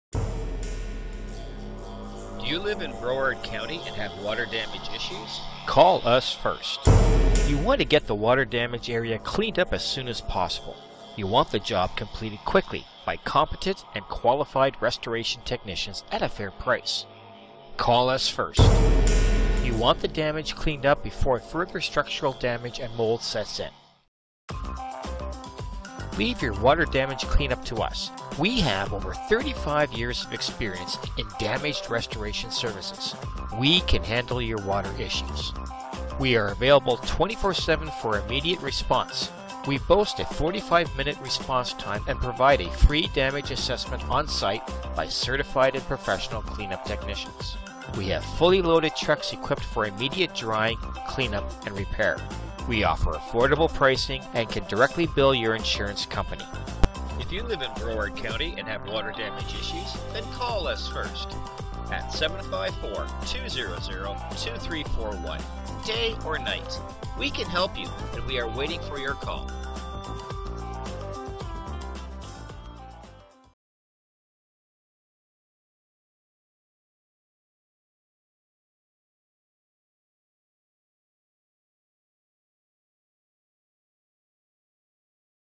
Broward County Water Damage Restoration Services
0:08  Your needs when you home or business is flooded.
0:19  Concerns over structural and mold damage due to floods
0:27  Leave your water damage concerns to us.
0:29  We have over 35 years of experience with damage restoration cleanup
0:37  We are available 24/7
0:40  We respond in 45 minutes
0:45  We provide FREE onsite damage assessment
0:50  We are fully equipped with trucks and technology to handle any damage cleanup.
0:54  We can work directly with your insurance company.
0:59  How to contact us for a free estimate.
1:15  South Florida areas served and types of services provided.

Call us first.  You want to get the water damaged area cleaned up as soon as possible. You want the job completed quickly, by competent and qualified restoration technicians and at a fair price. Call us first.
You want the damage cleaned up before further structural damage and mold sets in. Leave your water damage clean up to us. We have over 35 years of experience in damage restoration services. We can handle your water issues.
We are available 24/7 for immediate response, we boast a 45 minute response time and provide a FREE damage assessment onsite by certified and professional cleanup technicians. We have fully loaded trucks equipped to immediate drying, cleanup and repair. We offer affordable pricing and can directly bill your insurance companies.

All of our Water, Fire and Smoke damage restoration videos can be viewed on our channel